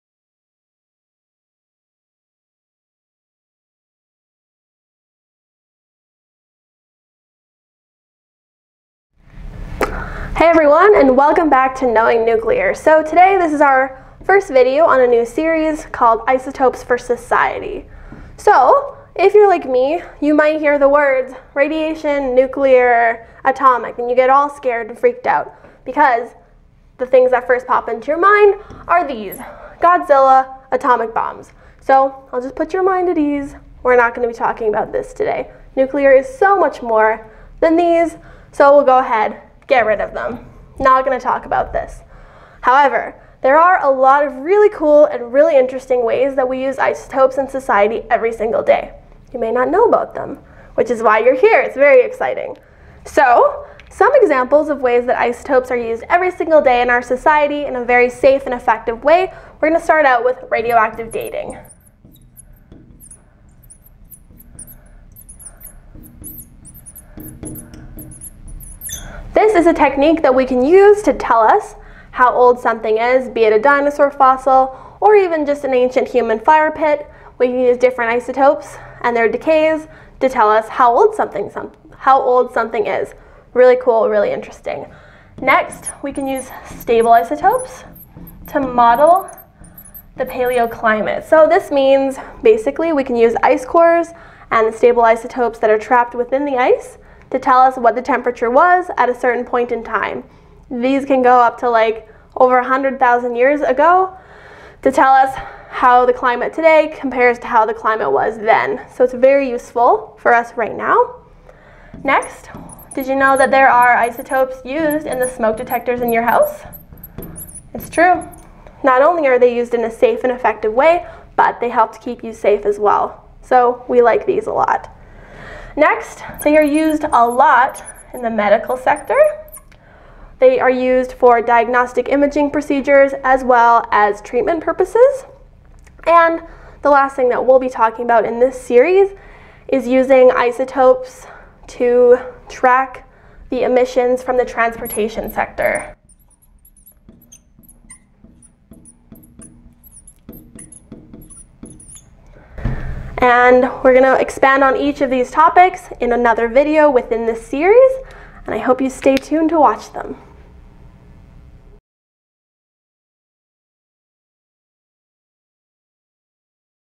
Isotopes for Society: Overview
Welcome to a new series called: Isotopes for Society. In this video, we will briefly introduce how isotopes are used in various ways in our society.